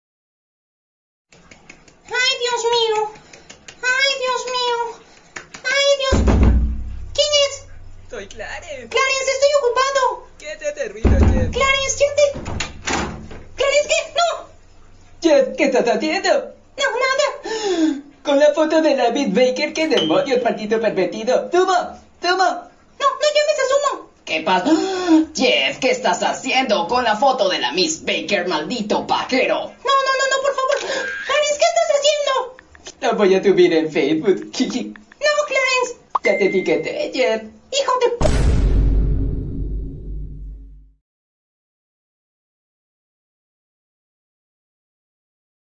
Jeff pierde el septiembre sin fap (prt 2)
jeff no aguanta y se hace una manuela

U n e t e 🗿

Remake de mí propia animación xdd